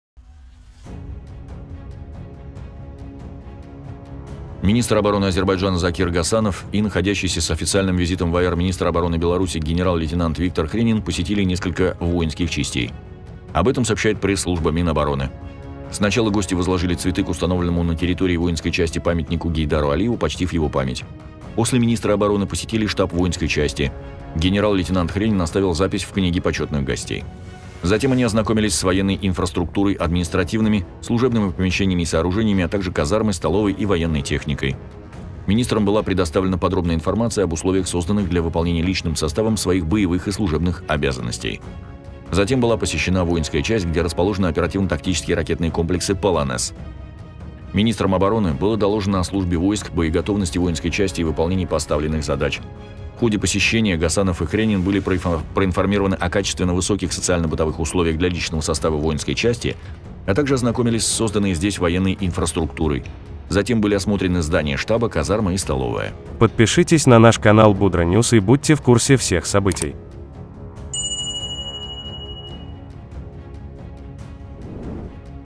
Министры обороны Азербайджана и Беларуси осмотрели «Полонезы»
Министр обороны Азербайджана генерал-полковник Закир Гасанов и находящийся с официальным визитом в нашей стране министр обороны Республики Беларусь генерал-лейтенант Виктор Хренин посетили несколько воинских частей.

Как сообщает пресс-служба оборонного ведомства, сначала гости возложили цветы к установленному на территории воинской части памятнику общенациональному лидеру азербайджанского народа Гейдару Алиеву, почтив его память.

После министры обороны посетили штаб воинской части. Генерал-лейтенант В.Хренин оставил запись в Книге почетных гостей. Затем министры ознакомились с военной инфраструктурой, административными, служебными помещениями и сооружениями, а также казармой, столовой и военной техникой. Министрам была предоставлена подробная информация об условиях, созданных для выполнения личным составом своих боевых и служебных обязанностей.

Затем была посещена воинская часть, где расположены оперативно-тактические ракетные комплексы «Полонез». Министрам обороны было доложено о службе войск, боеготовности воинской части и выполнении поставленных задач. В ходе посещения министры были проинформированы о качественно высоких социально-бытовых условиях для личного состава воинской части, а также ознакомились с созданной здесь военной инфраструктурой. Затем были осмотрены здание штаба, казарма и столовая.

В заключение министры обороны посетили парк хранения оперативно-тактических ракетных комплексов «Полонез», склад хранения боевых ракет и центр технического обслуживания.